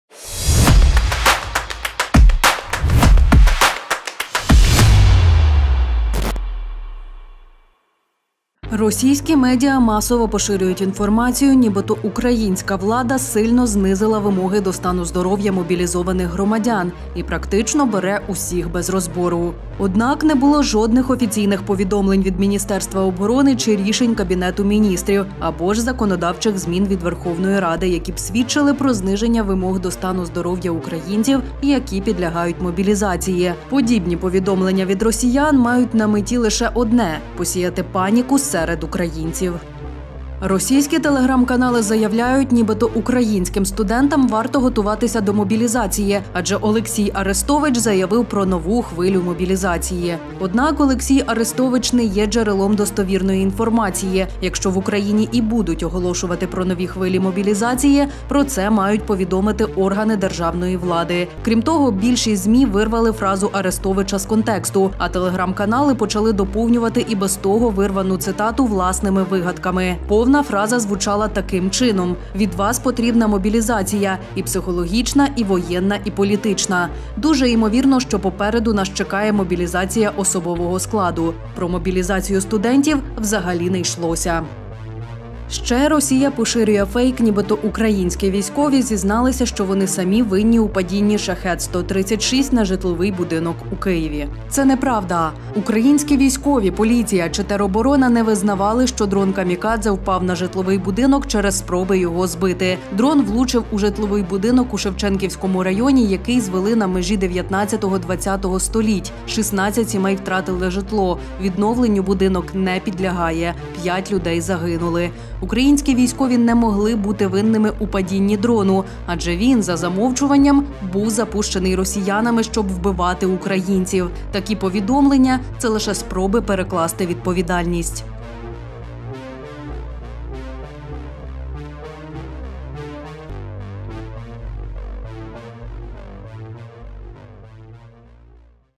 Українські студенти готуються до мобілізації та інші фейки. VoxCheck
Цього разу російська пропаганда збрехала про:
- зниження вимог до стану здоров’я українців, які підлягають мобілізації
- мобілізацію студентів
- падіння Shahed-136